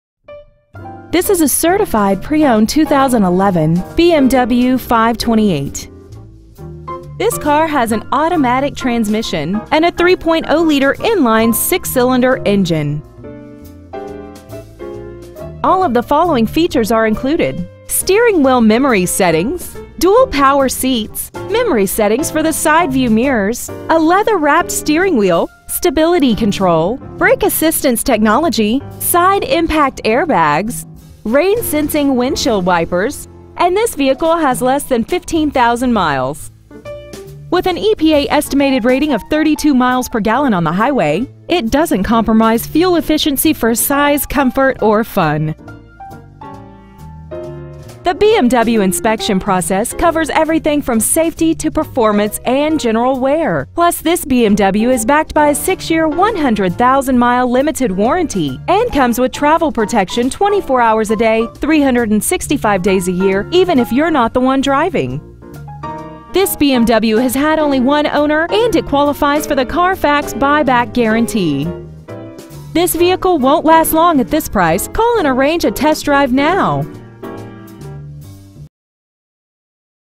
2011 BMW 528 Certified Monrovia CA 91016
Year : 2011
 Make : BMW
 Model : 528
 Engine : 3.0L I-6 cyl
 Trans . : Automatic
 Exterior : Black
 Miles : 14,662
 Interior : Black
 BMW of Monrovia
 2011 BMW 528 Monrovia CA